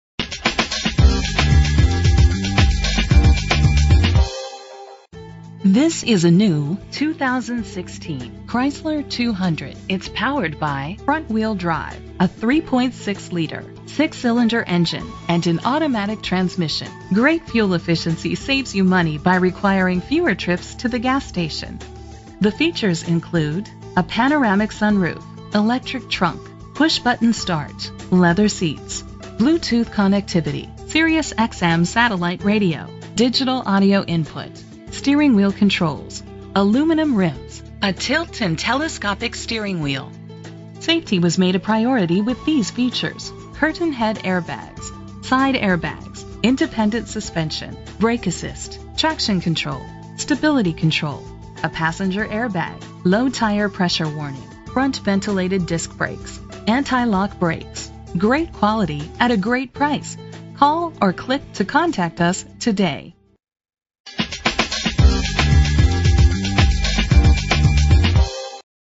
2016 Chrysler 200 - ORLANDO FL
Year: 2016
Make: Chrysler
Model: 200
Trim: S
Engine: 3.6 liter 6 cylinder 24 valve
Transmission: 9-Speed Automatic
Color: Granite Crystal Clearcoat Metallic
Mileage: 10
Address:
9051 EAST COLONIAL DRIVE